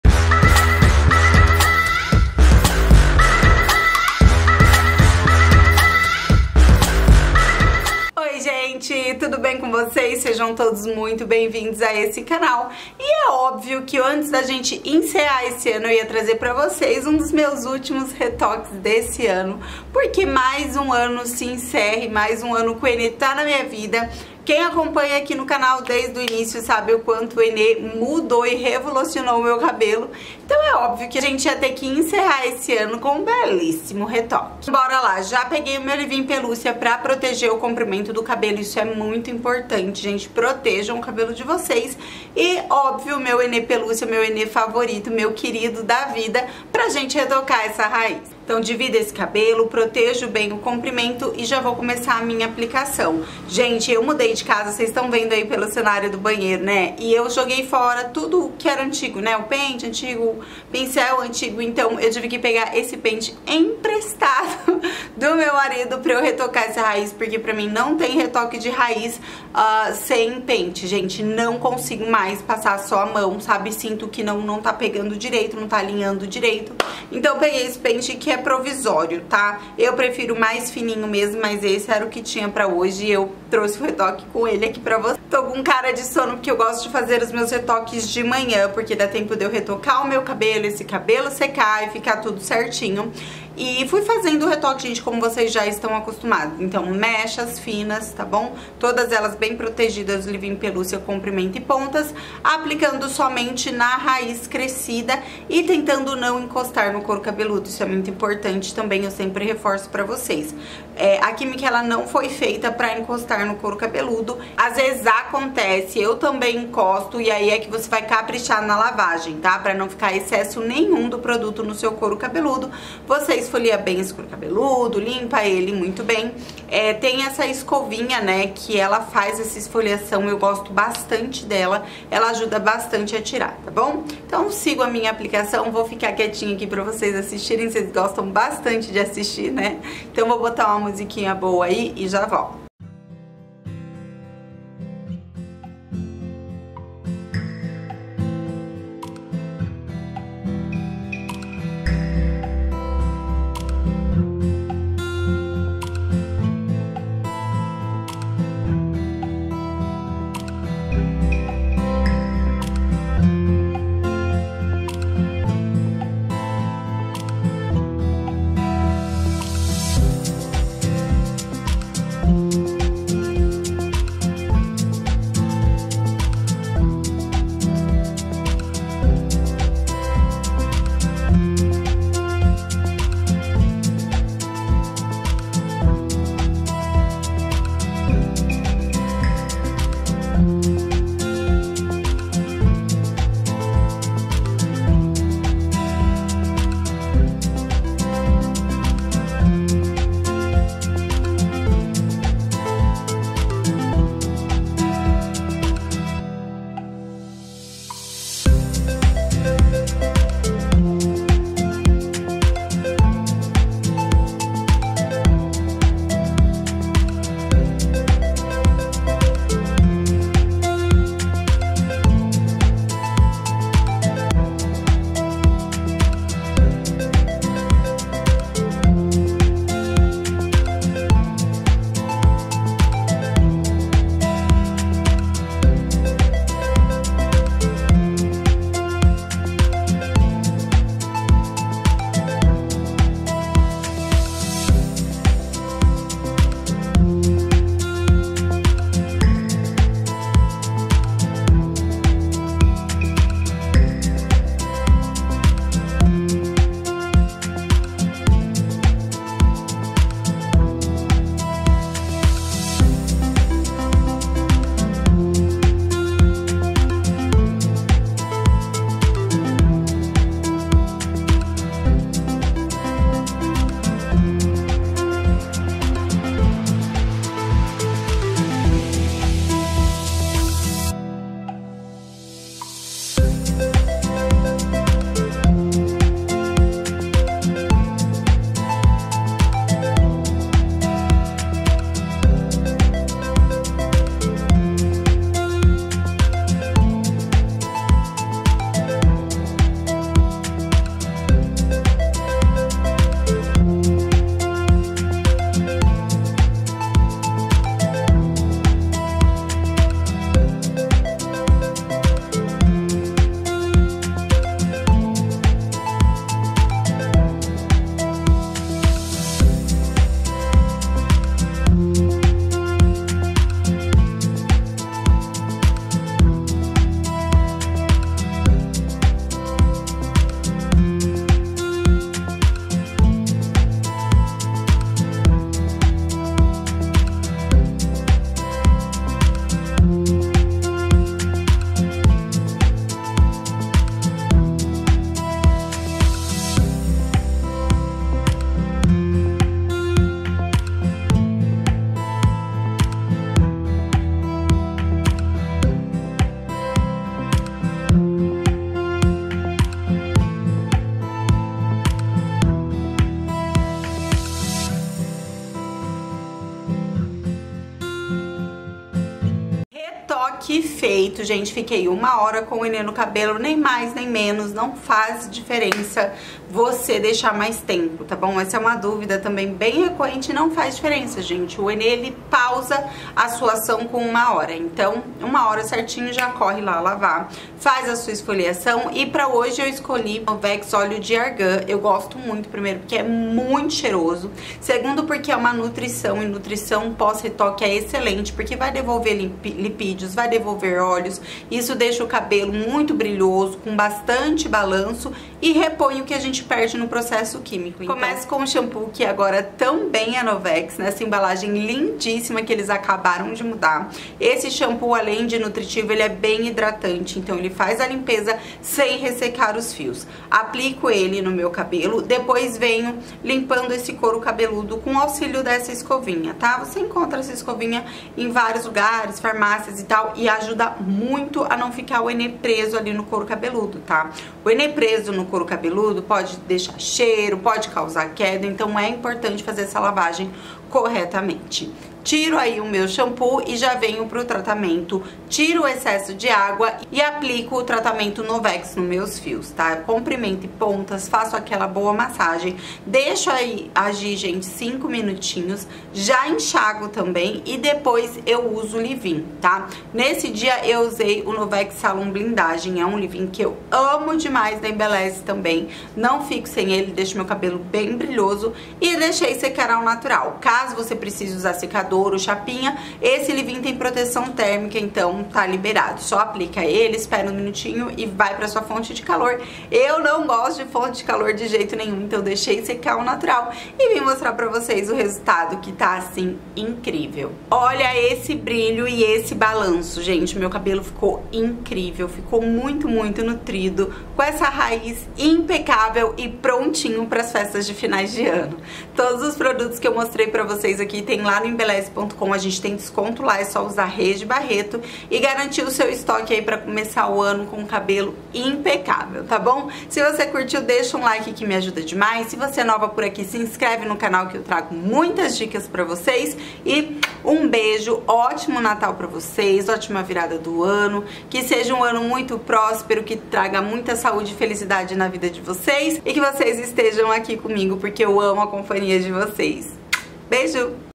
retoque de Natal
Oi Lindonas, bora de retoqe de raiz com henê pro final de ano.
Desejo  a vcs um otimo Natal e um prodpero ano novo,obrigada por estarem aqui comigo.